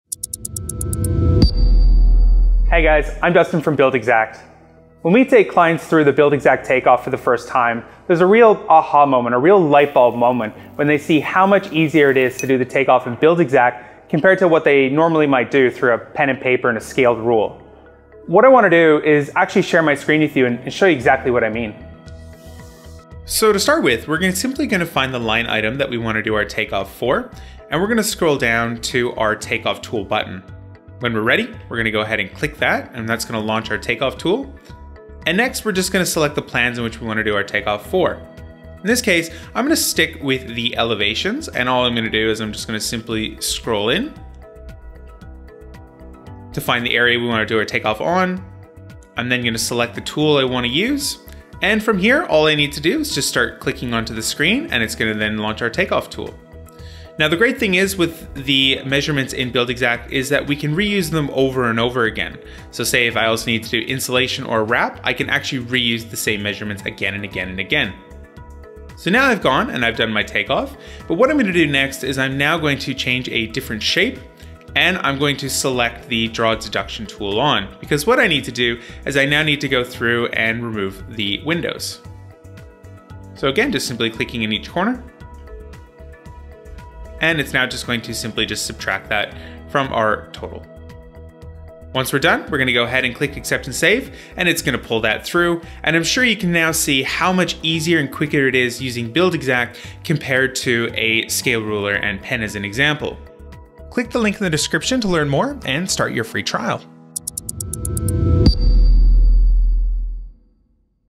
Learn How to do Takeoffs Faster with Buildxact's Software
Check out how easy it is to do a takeoff with Buildxact, an ideal estimating and project management solution for residential builders and contractors. No more pens and paper, simply drag and drop to select an area to measure.

Sign up to a free 14-day trial to see if Buildxact is the right fit for your business

estimation and project management has never been so easy.